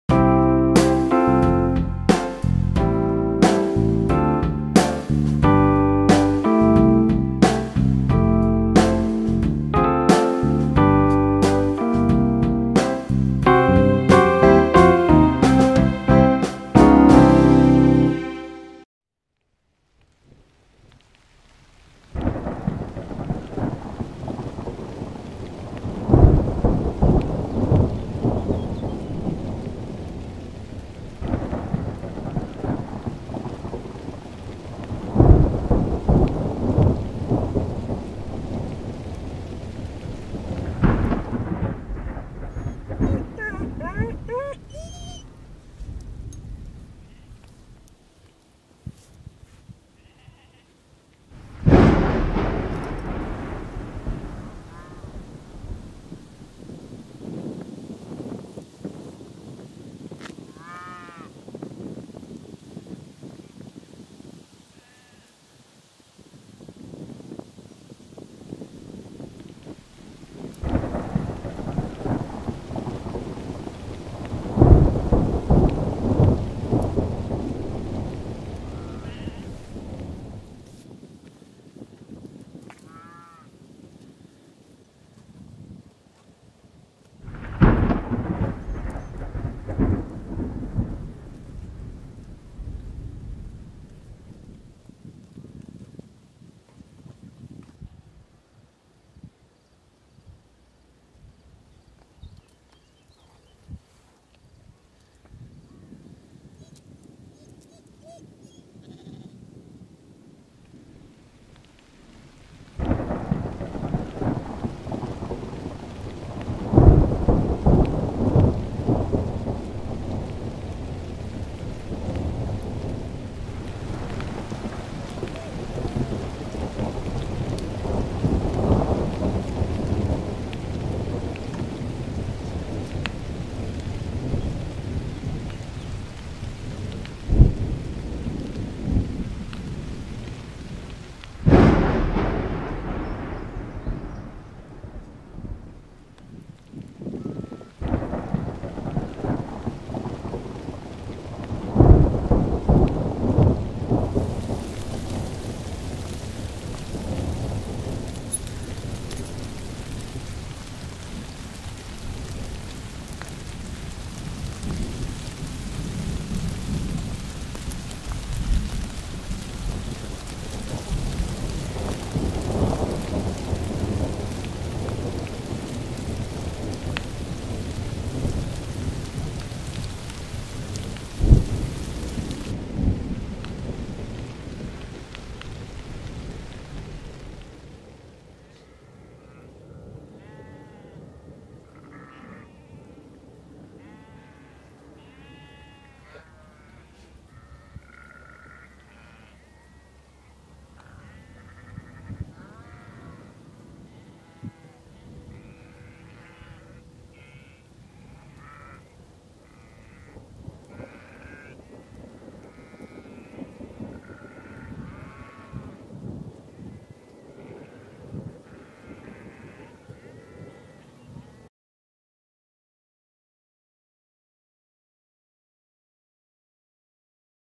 Die Schafe sind zurück, Eichelberg 2011
Jedes Jahr im Frühjahr und Herbst erscheint auf dem Eichelberg bei Bruchsal eine tausendköpfige Schafherde.